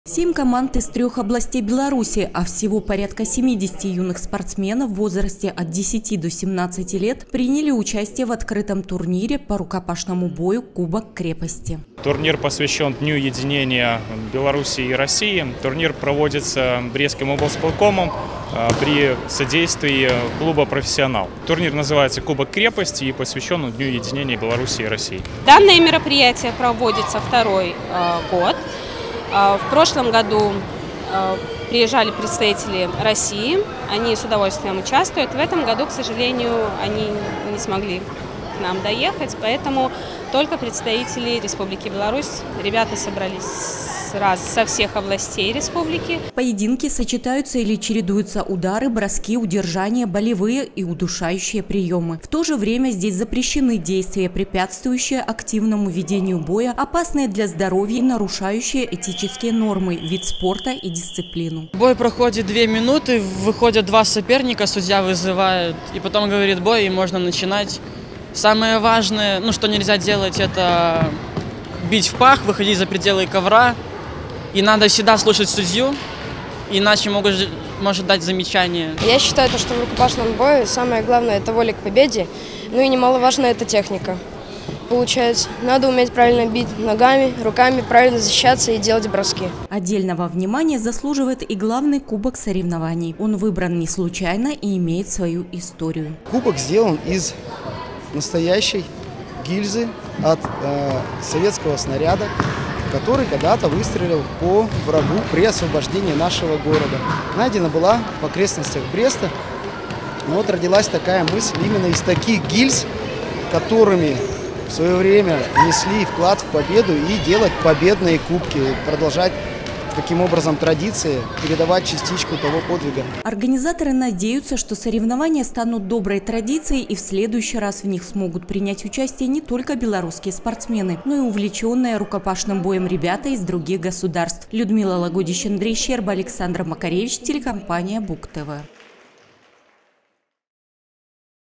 2022-04-07 г. Брест. Открытый турнир по рукопашному бою «Кубок крепости». Новости на Буг-ТВ.
Кубок крепости»  - открытый турнир по рукопашному бою под таким названием прошел в нашем городе. Его организаторами стали Главное управление идеологической работы и по делам молодёжи Брестского облисполкома при поддержке Русского дома в Бресте и клуба «Профессионал».  Мероприятие приурочили ко дню единения Беларуси и России, оно получилось зрелищным и ярким.